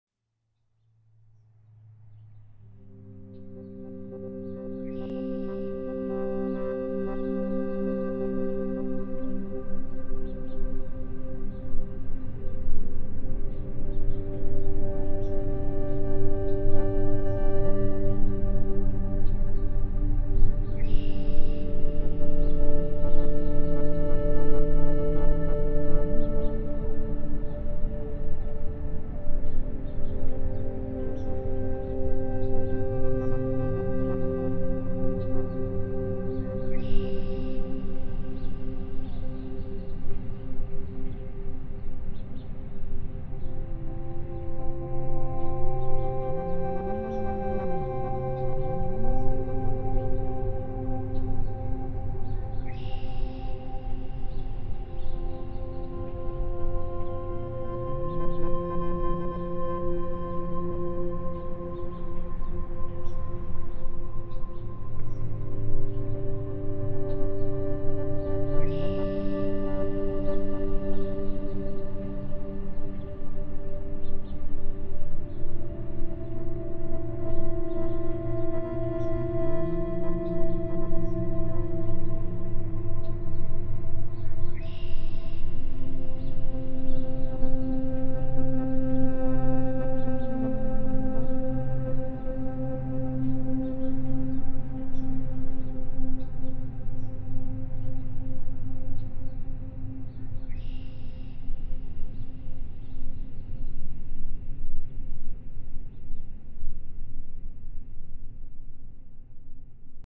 Dark Sun Birds
Shot on the Riverwalk in Lowell, Massachusetts

Created entirely with Reaper